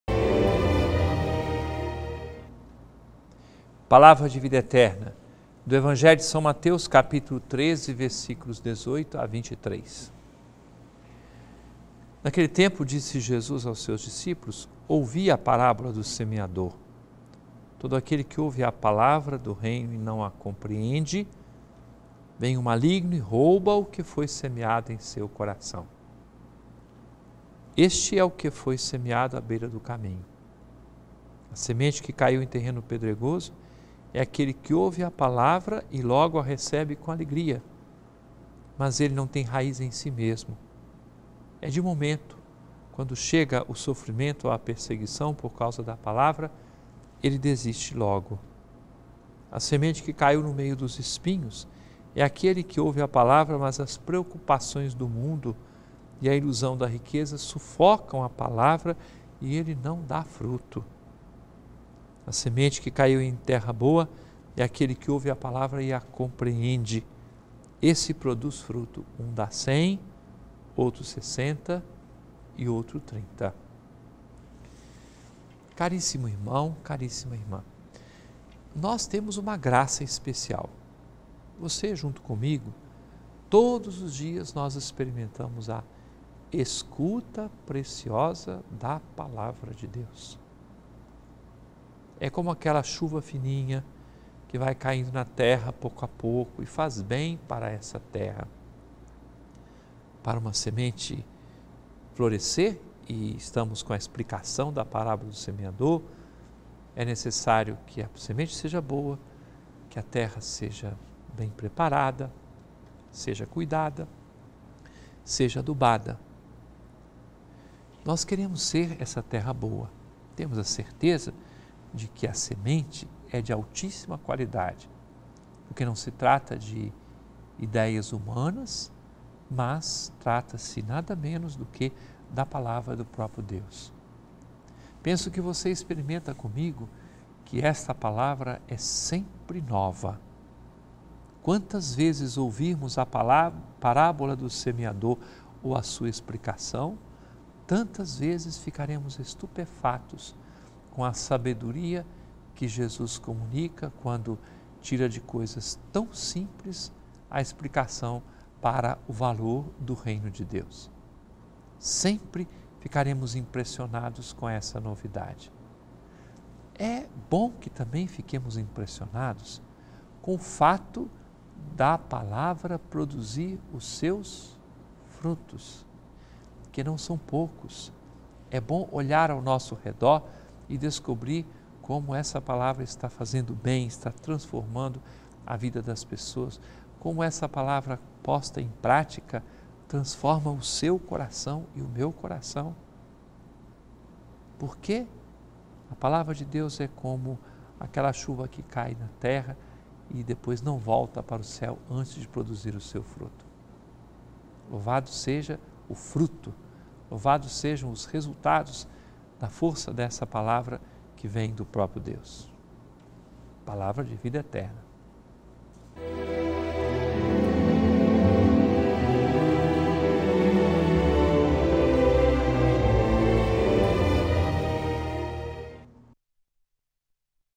Mateus 13,18-23 - 27/07/2012
Naquele tempo, disse Jesus aos seus discípulos: 18Ouvi a parábola do semeador: 19Todo aquele que ouve a palavra do Reino não a compreende, vem o Maligno e rouba o que foi semeado em seu coração. Este é o que foi semeado à beira do caminho. 20A semente que caiu em terreno pedregoso é aquele que ouve a palavra e logo a recebe com alegria;
21mas ele não tem raiz em si mesmo, é de momento: quando chega o sofrimento ou a perseguição, por causa da palavra, ele desiste logo. 22A semente que caiu no meio dos espinhos é aquele que ouve a palavra, mas as preocupações do mundo e a ilusão da riqueza sufocam a palavra, e ele não dá fruto. 23A semente que caiu em boa terra
é aquele que ouve a palavra e a compreende. Esse produz fruto. Um dá cem, outro sessenta e outro trinta.'